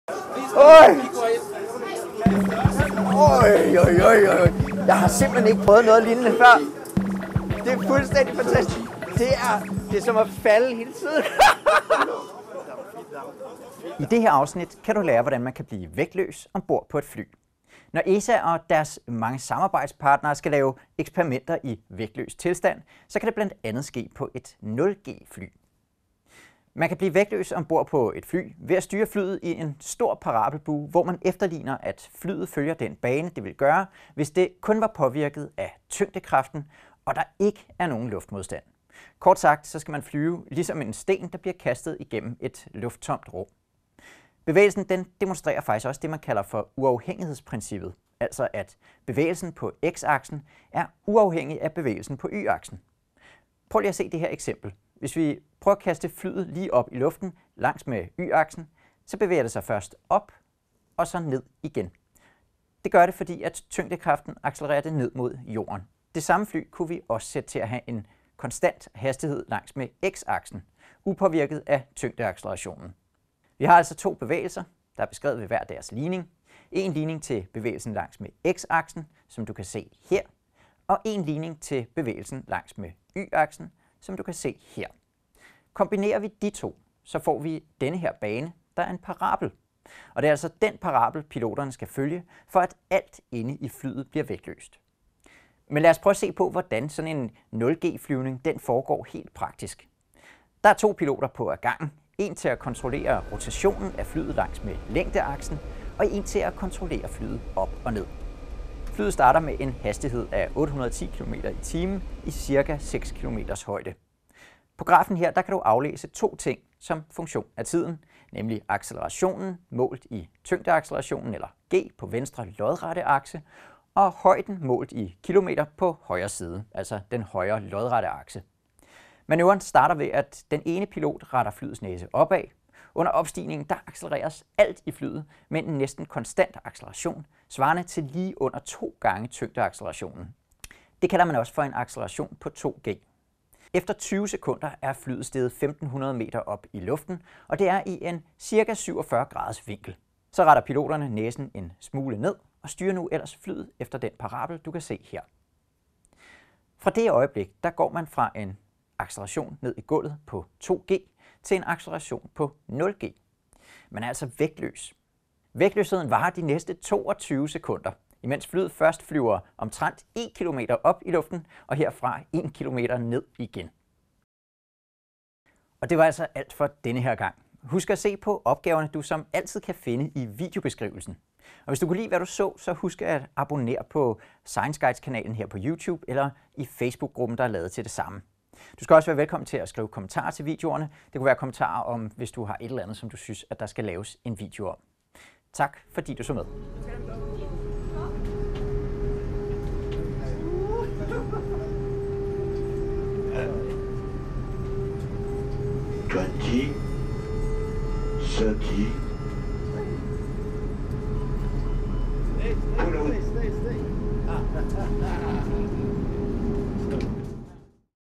Fysik: Vægtløs på et zero-G fly
I denne video gennemgår vi hvordan man kan blive vægtløs ombord på et fly af typen Airbus A300. Denne video er 4. film i en serie på 4.

Tak til:
ESA samt Novespace